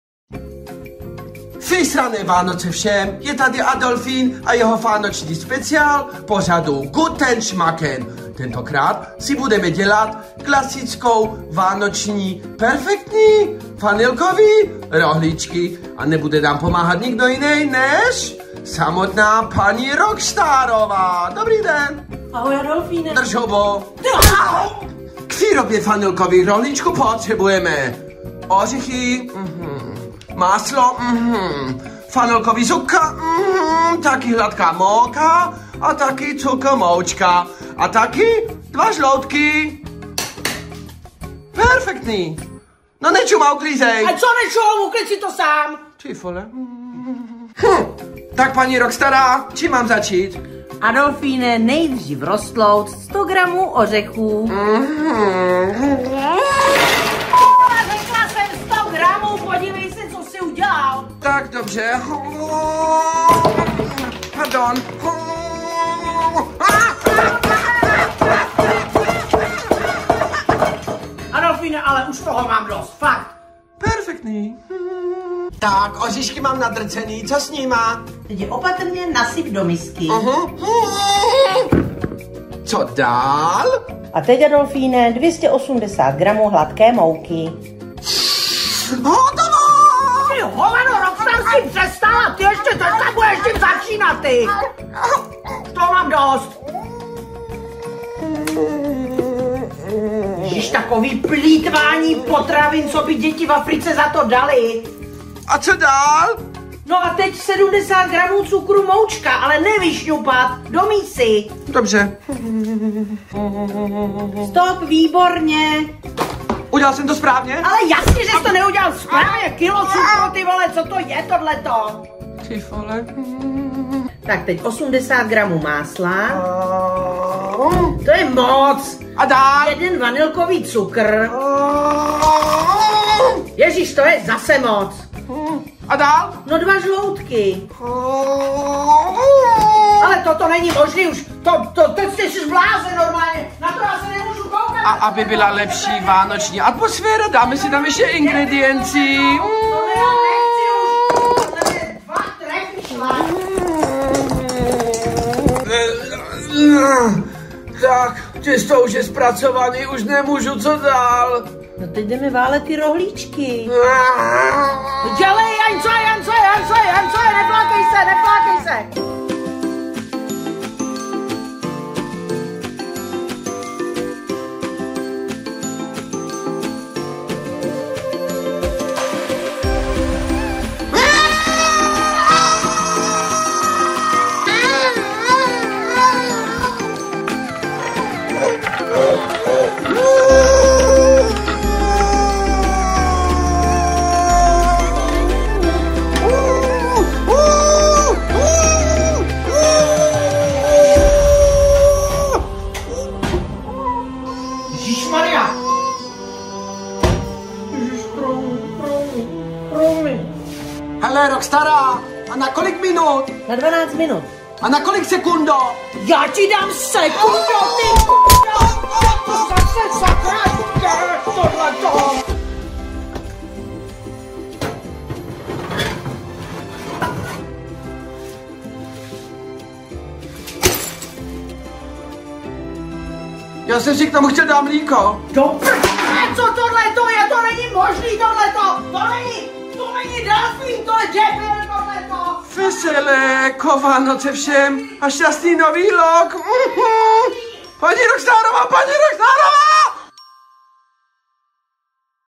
ADOLFEEN - FÁNOČNÍ RECEPT NA FANILKOFÉ ROHLÍČKY
Tentokrát Adolfeen fařil s Roxtárofou mámou a nebylo jí to jedno :D